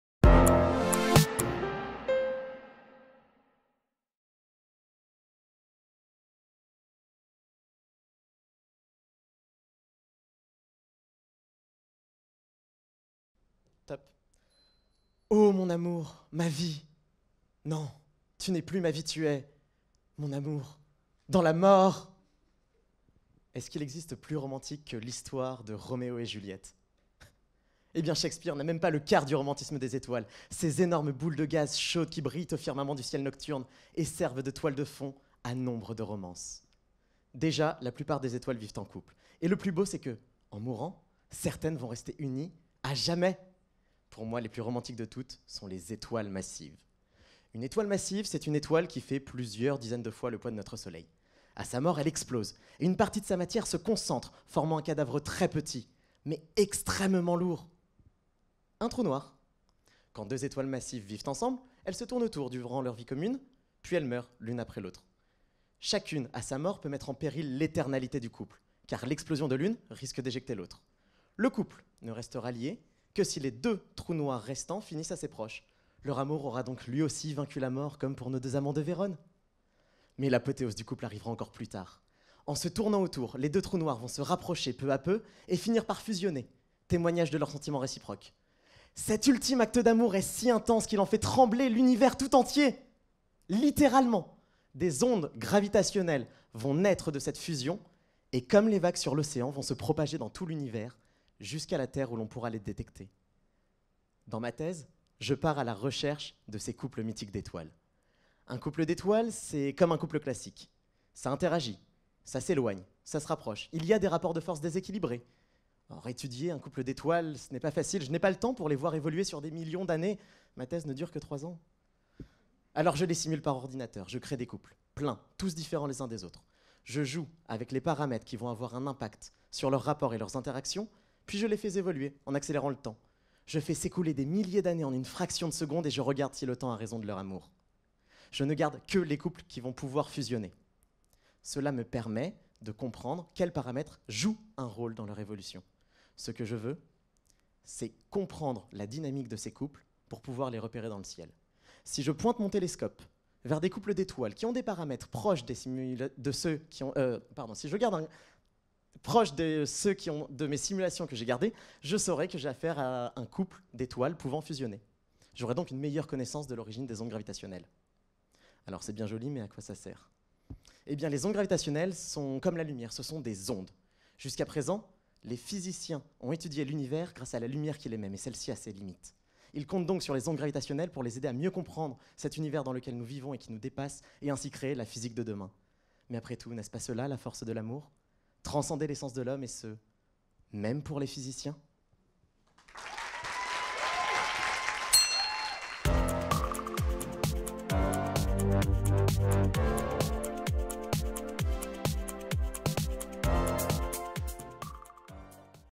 Julien Marchioro- - Prix du public MT180
Julien Marchioro, inscrit dans l’école doctorale 560, Step Up, a présenté son travail de thèse intitulé : Évolution de systèmes binaires massifs et impact des paramètres évolutifs sur la formation de binaires d’objets compacts.